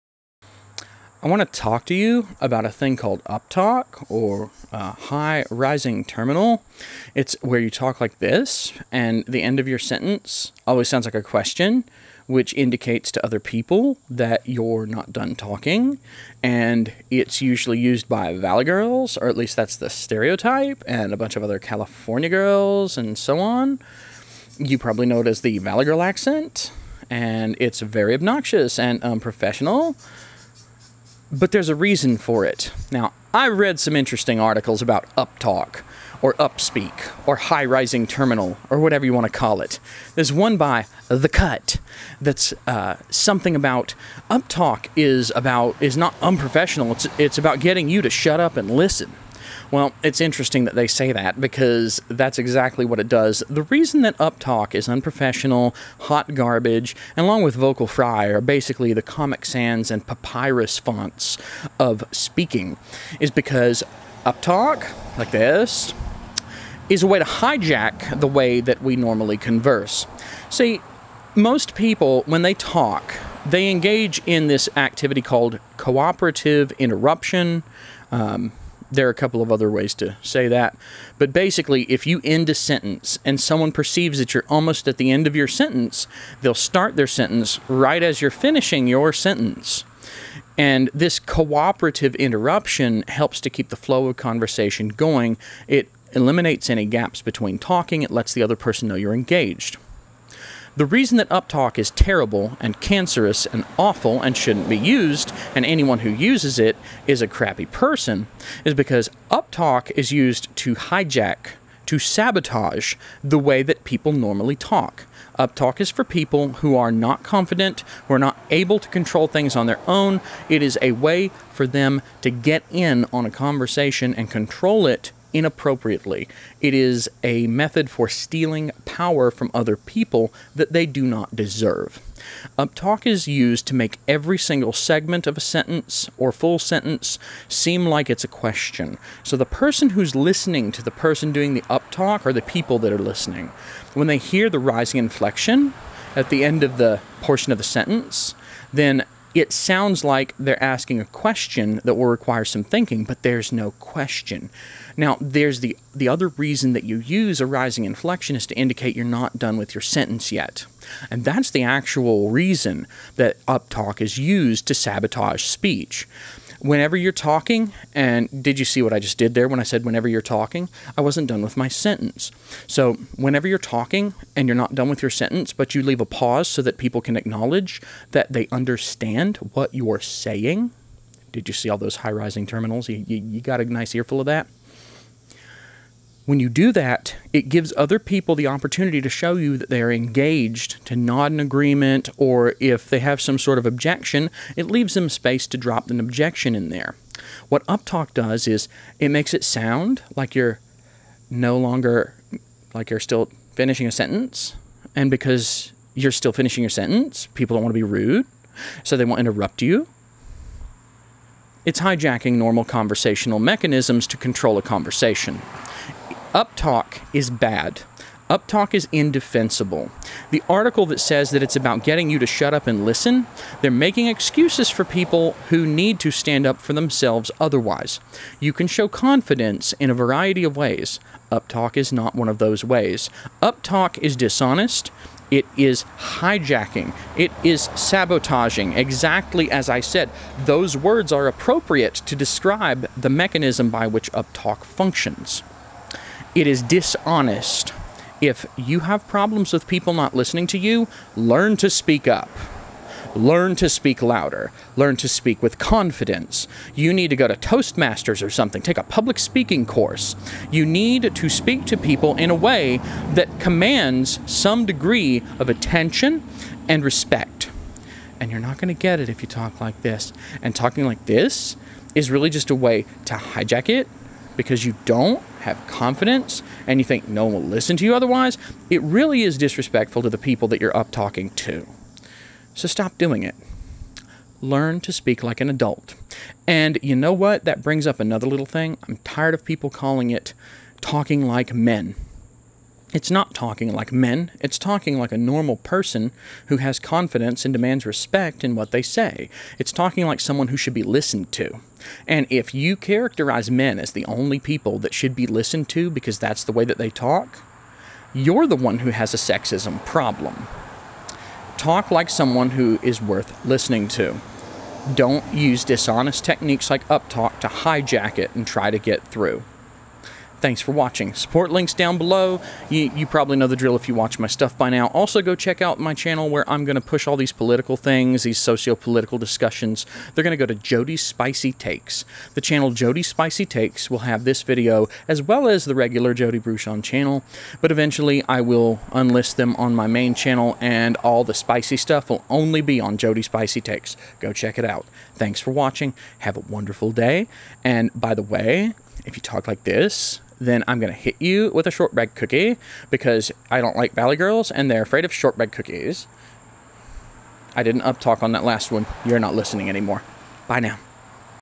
Uptalk is Dishonest, Obnoxious Conversational Hijacking - High Rising Terminal - Jody Bruchon
If uptalk is the only way you can get people to listen to you, you don't deserve to be heard. The high rising terminal is used to indicate that a sentence being spoken is incomplete or to give a pause for others to show that they're listening and engaged, but it is also used (particularly by unconfident women) to prevent others from engaging in the healthy and natural practice of cooperative interruption during conversation. This hijacking of a natural conversational mechanism to instead dominate the conversation is both dishonest and annoying, and should be shunned whenever encountered. It's not unprofessional because society hates women, it's unprofessional because it's a deceptive behavior--basically, an unconscious lie.

H I G H  R I S I N G  T E R M I N A L